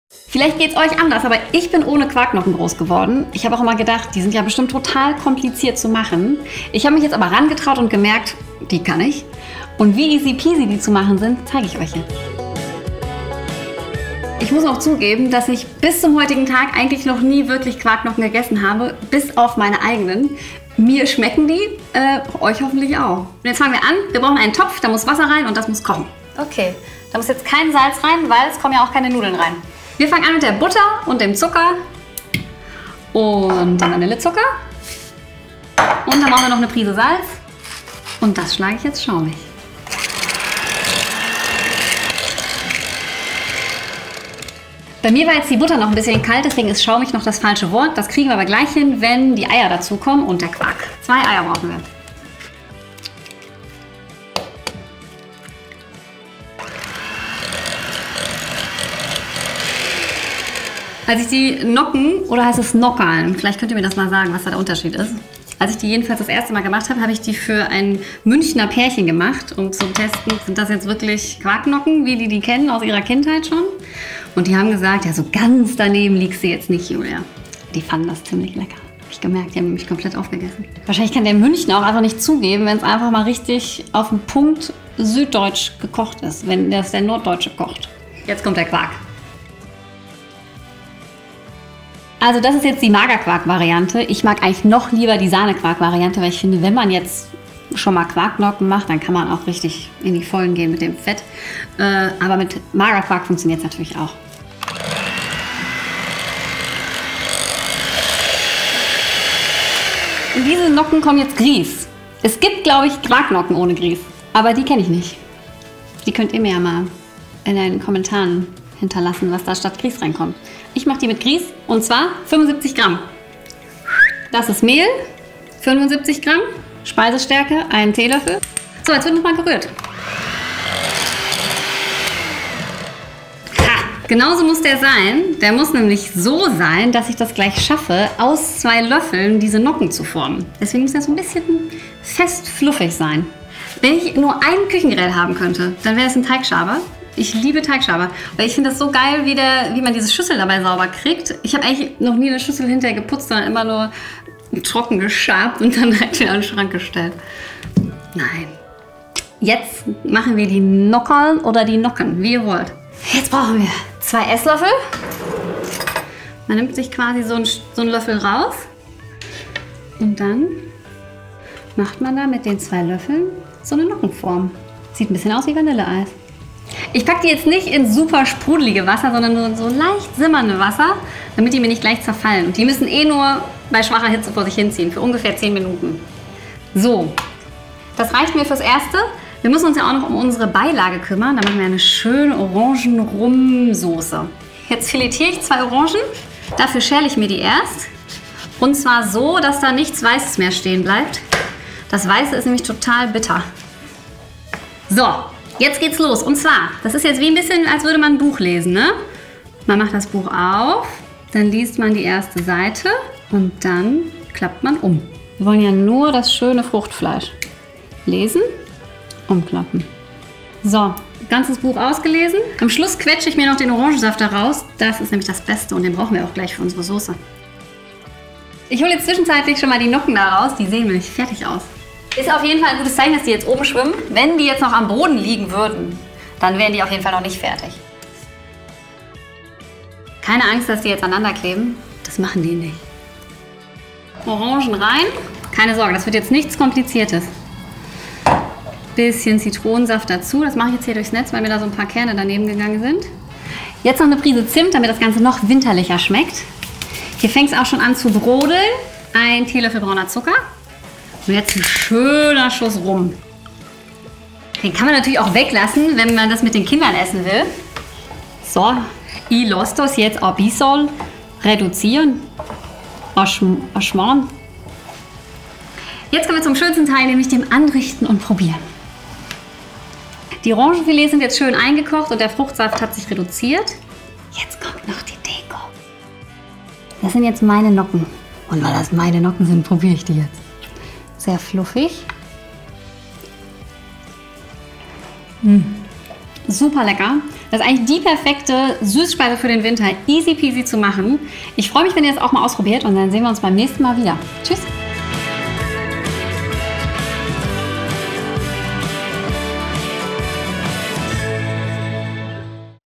Quarknocken mit Orangenfilets Rezept I Winterliche Süßspeise | EDEKA
Das perfekte Dessert für kalte Wintertage: Quarknocken mit karamellisierten Orangenfilets. Julia zaubert hier aus Quark, Zucker, Butter, Ei und Grieß luftig-lockere Quarknocken und lässt auch beim Topping keine Wünsche offen. Die fruchtigen Orangenfilets werden karamellisiert und mit einem ordentlichen Schuss Rum verfeinert – lecker!  Das ist definitv ein winterliches Dessert, das glücklich macht. Probiert es aus!

Euch können die Nocken locken? Dann geht’s hier zum Rezept